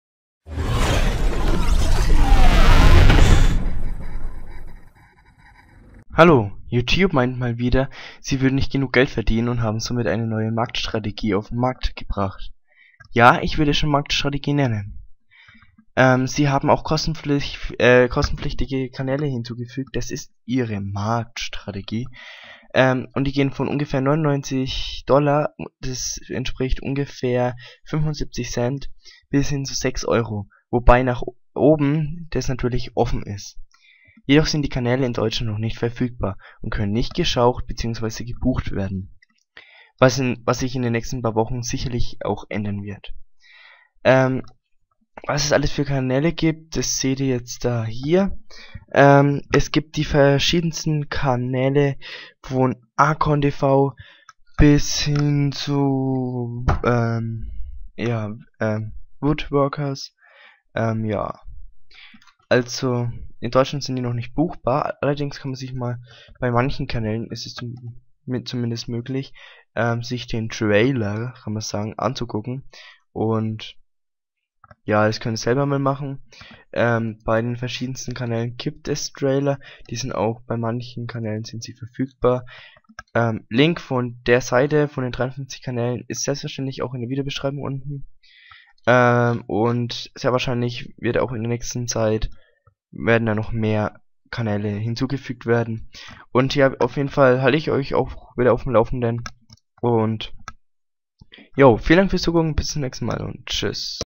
YouTube wird kostenpflichtig?!
In diesem Video wird gezeigt, dass YouTube 53 Kanäle veröffentlicht hat, welche ab sofort kostenpflichtig sind.